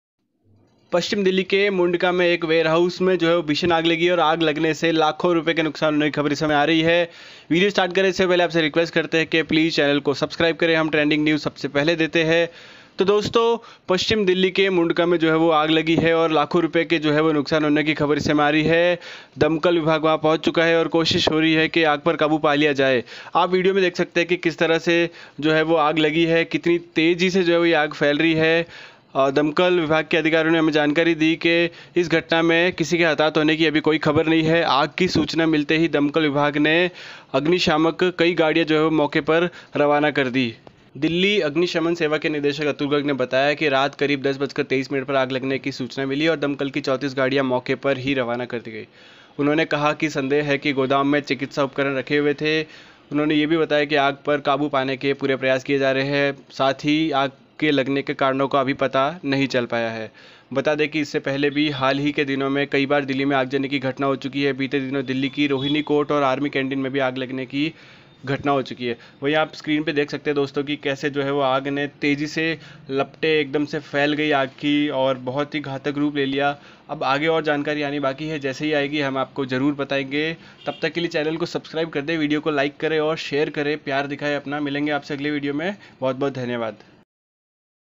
Mundka Warehouse Fire Broke Out | West Delhi's Mundka Warehouse Turned Into Fire
Major fire in West Delhi warehouse: Official

It is suspected that the warehouse had medical equipments, he said, adding the fire-fighting operations are underway to bring the blaze under control.

A major fire broke out at a warehouse in West Delhi’s Mundka area on Wednesday night, a Delhi Fire Service official said.

No casualty has been reported so far, he said.

A call about the fire was received at around 10.23 pm and 34 fire tenders were rushed to the spot, said Atul Garg, Delhi Fire Service Director.

It is suspected that the warehouse had medical equipments, he said, adding the fire-fighting operations are underway to bring the blaze under control. The cause of fire is not known yet, he added.

Major fire breaks out in West Delhi warehouse, no casualties reported so far: Official